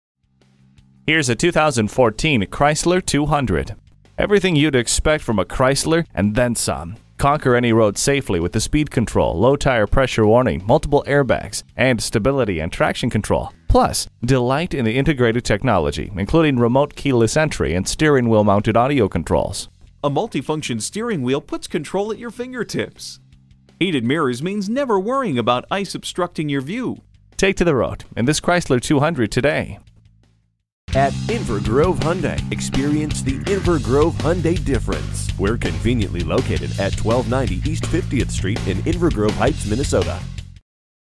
2014 Chrysler 200 Minneapolis MN St Paul, MN P7781 - SOLD
Year: 2014
Make: Chrysler
Model: 200
Trim: 4dr Sdn LX
Engine: 2.4 L 4 Cyl. Cyl.
Transmission: Automatic
Color: Crystal Blue Pearlcoat
Interior: Lt Frost Beige/Black
Mileage: 37887
Stock #: P7781
VIN: 1C3CCBAB9EN142118

Address:
1290 E 50th Street